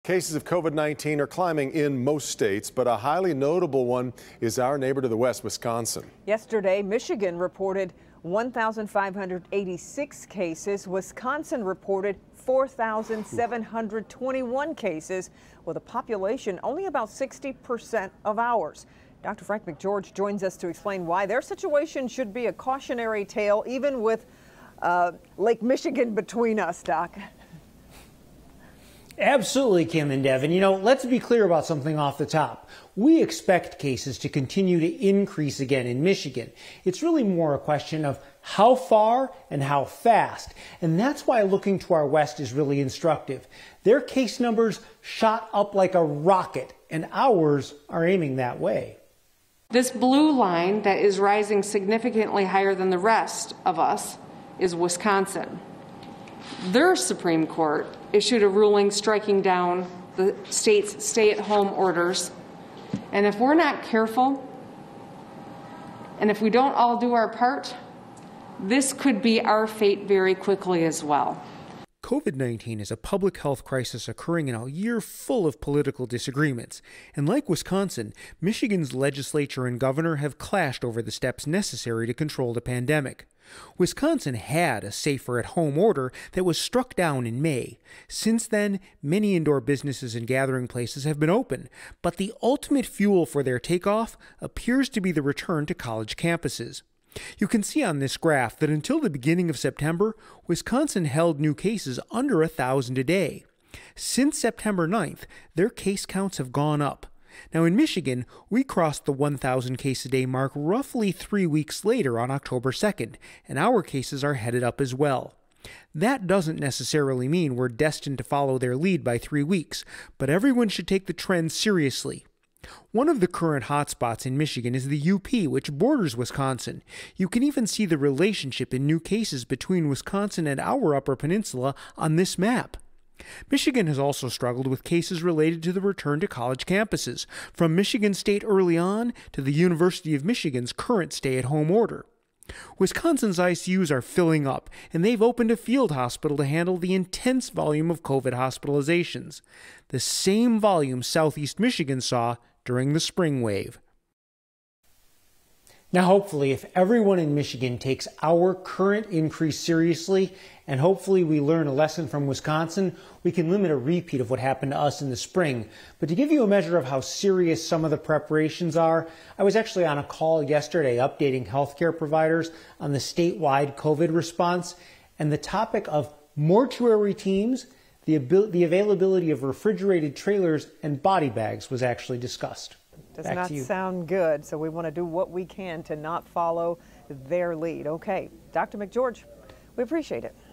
Worsening coronavirus pandemic in Wisconsin possible forecast for Michigan
Experts believe worsening coronavirus conditions in Wisconsin might be replicated in Michigan is action is not taken as COVID-19 cases surge.